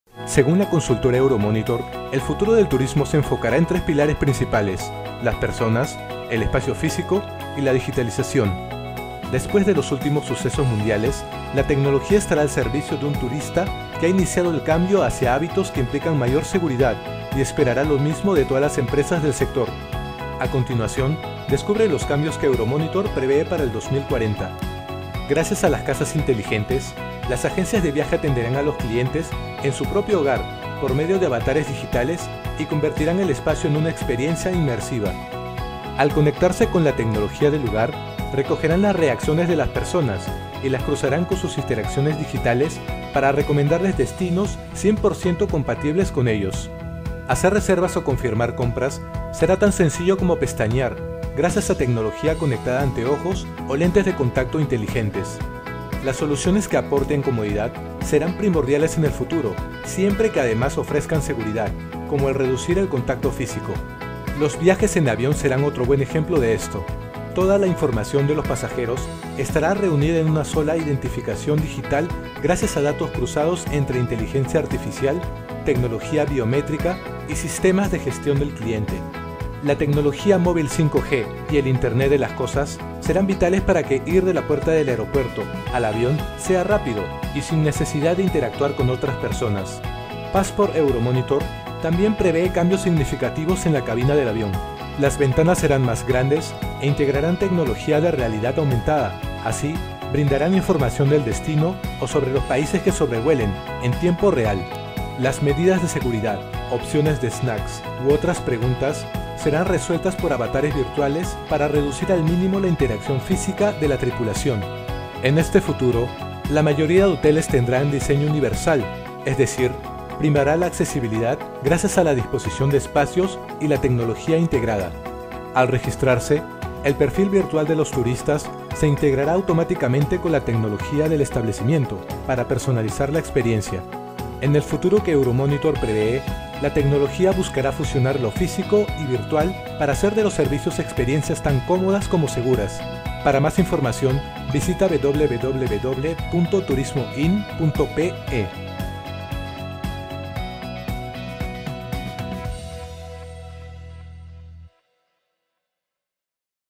¿Cómo será el turismo en el futuro?
TurismoIn | Descubre cuáles serán los pilares clave en el sector durante las siguientes décadas, cómo se integrará la tecnología al servicio turístico y qué demandarán las personas.